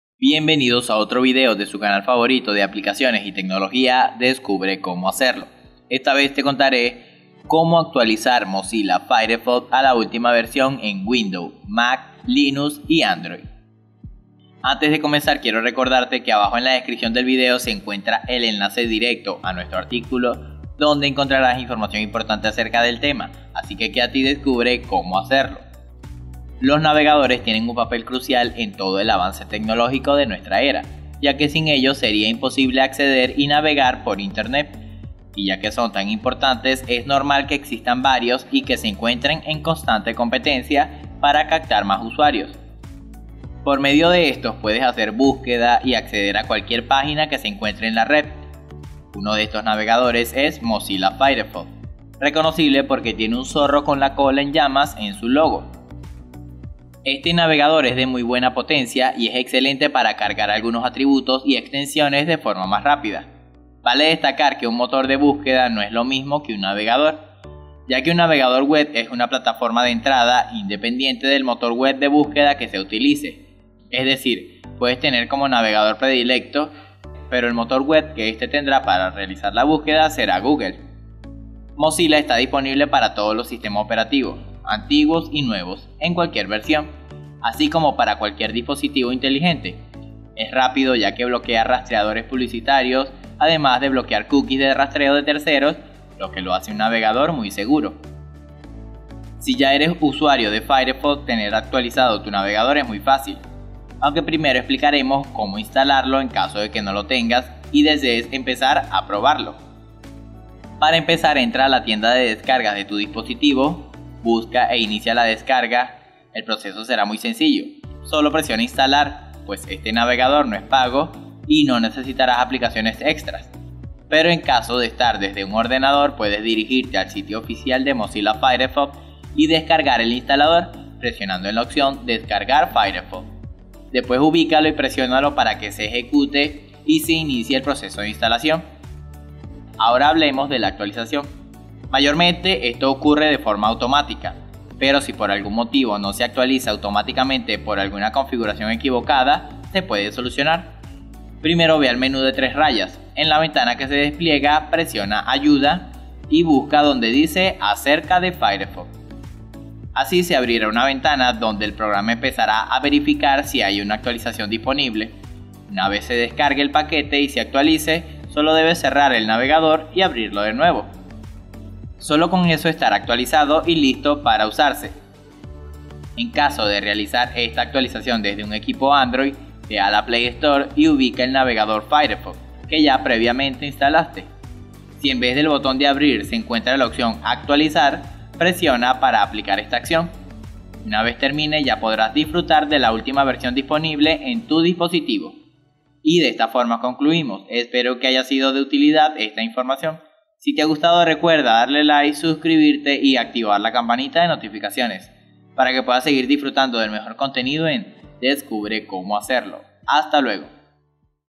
Cómo Actualizar Mozilla Firefox a la Última Versión en Windows, Mac, Linux y Android
En este video descubre Cómo Actualizar Mozilla Firefox a la Última Versión en Windows, Mac, Linux y Android. En el Canal Descubre Cómo Hacerlo encontrarás el mejor contenido. ¡No te lo puedes perder!